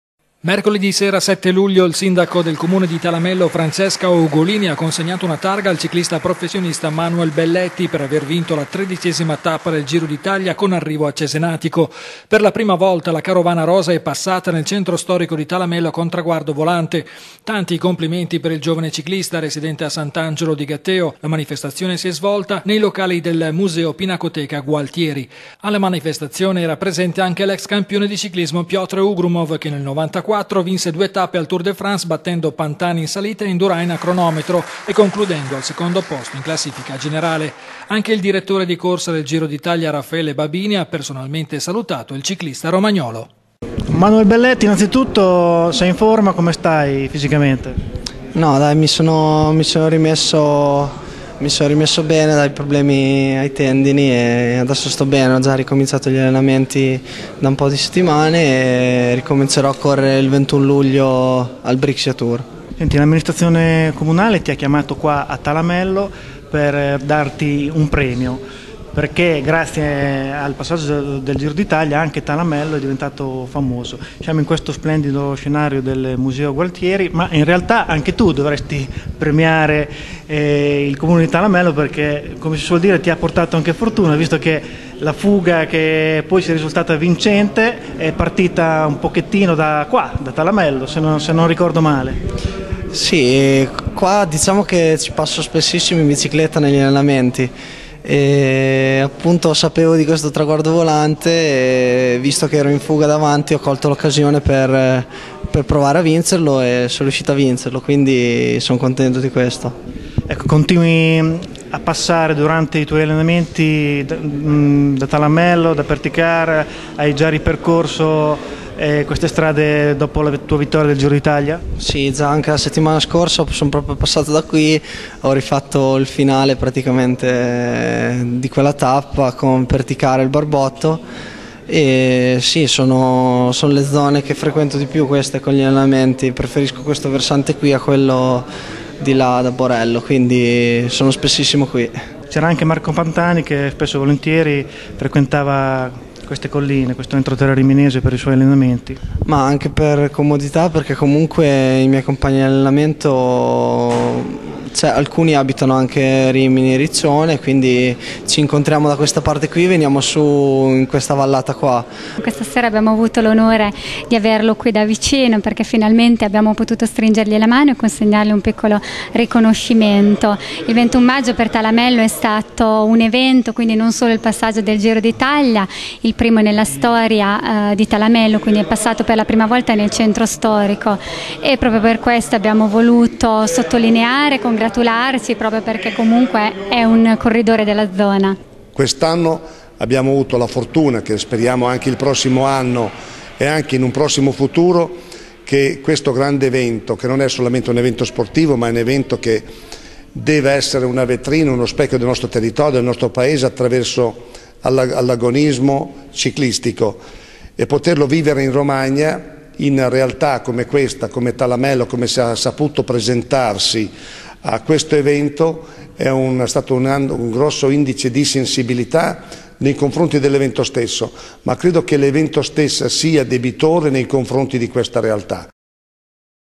Il ciclista Manuel Belletti premiato a Talamello
Mercoledi sera 7 luglio il sindaco del Comune di Talamello, Francesca Ugolini ha consegnato una targa al ciclista professionista Manuel Belletti, per aver vinto la 13esima tappa del Giro d'Italia con arrivo a Cesenatico. Per la prima volta la carovana rosa è passata nel centro storico di Talamello con traguardo volante. Tanti i complimenti per il giovane ciclista, residente a Sant'Angelo di Gatteo. La manifestazione si è svolta nei locali del Museo pinacoteca Gualtieri. Alla manifestazione era presente anche l'ex campione di ciclismo Piotr Ugrumov che nel 1994 vinse due tappe al Tour de France, battendo Pantani in salita e Indurain a cronometro, e concludendo al secondo posto in classifica generale. Anche il direttore di corsa del Giro d'Italia Raffaele Babini ha personalmente salutato il ciclista romagnolo.